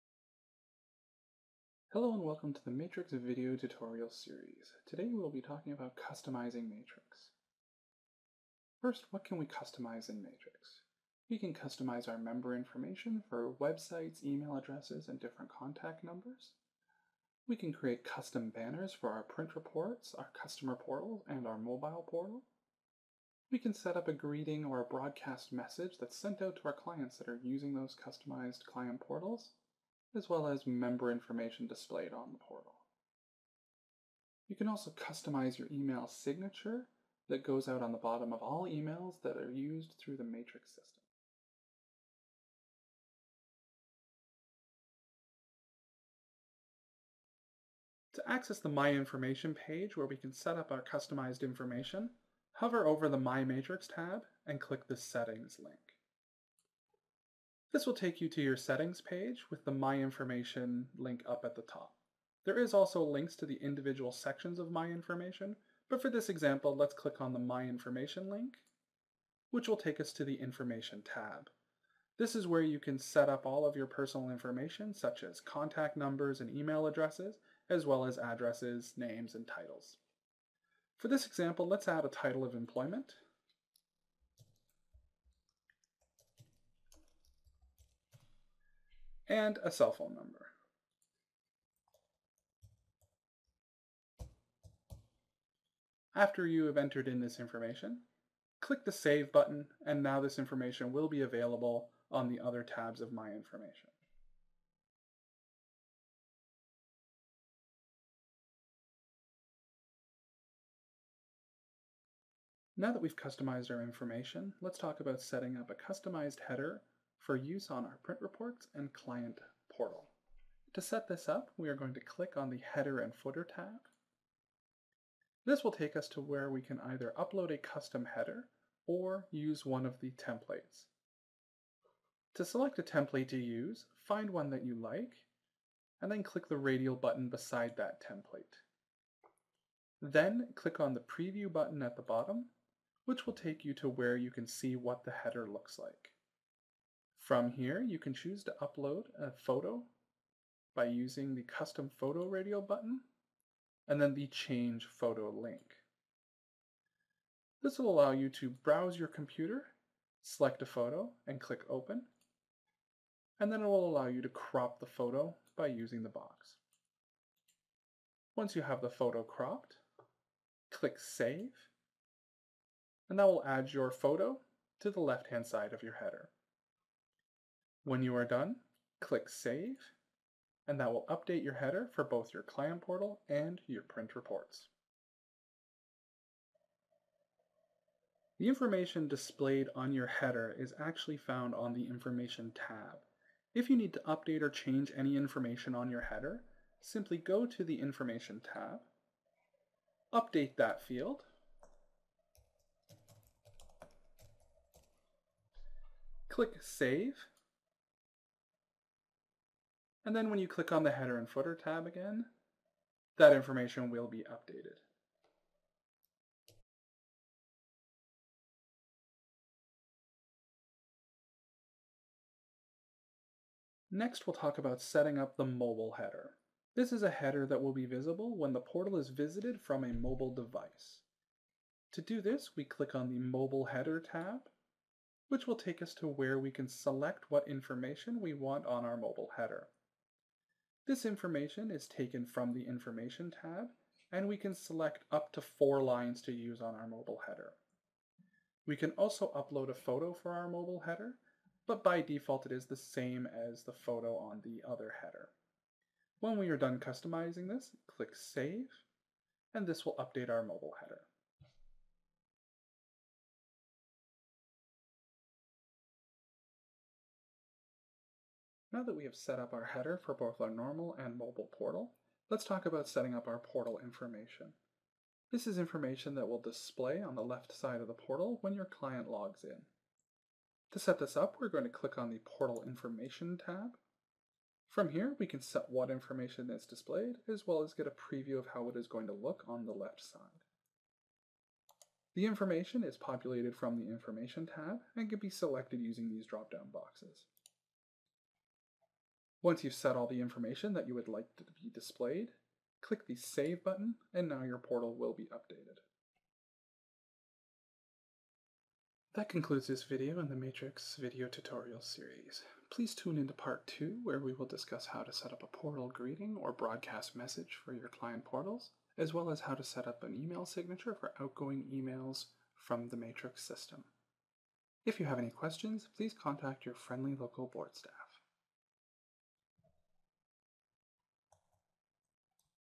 Matrix™ v6.12 - Customize (Part 1)
Video tutorial on customizing Matrix.

Prepared by The Oakville, Milton and District Real Estate Board for the Ontario Regional Technology and Information Services.